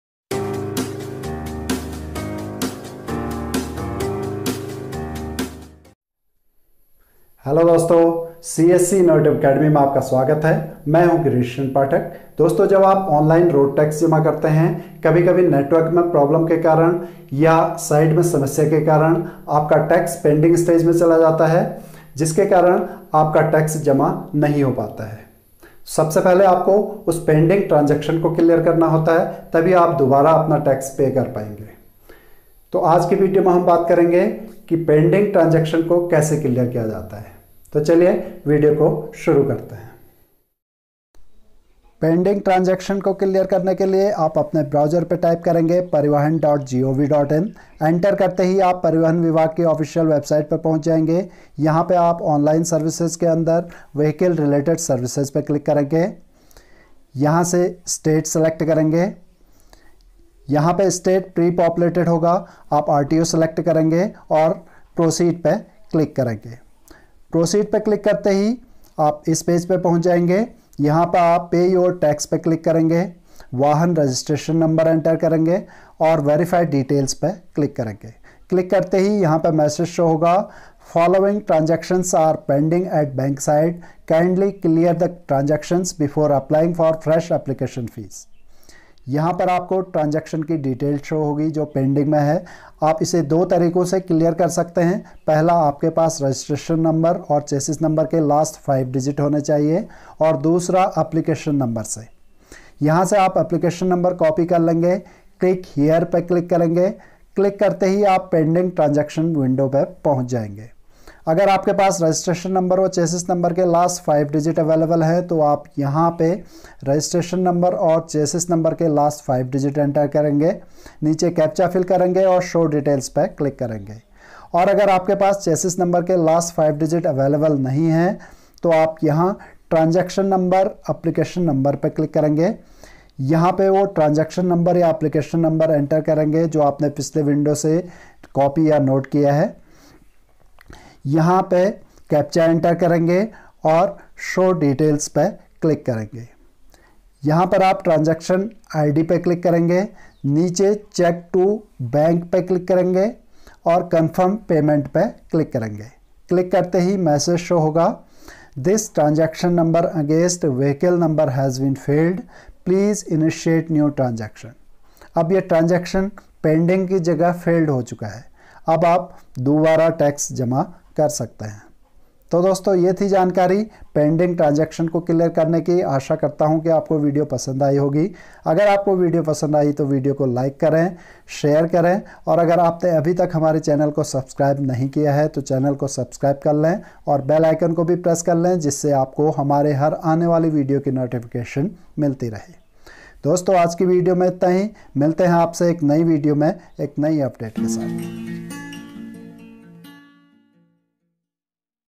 How to clear pending transaction/ Vahan Tax pending transaction kaise clear kare?
When you pay tax for any vehicle sometimes tax goes pending at bank side. you first have to clear the pending transaction before initiating new transaction. If you ever come across the same problem, watch this video it gives you complete solution for the same.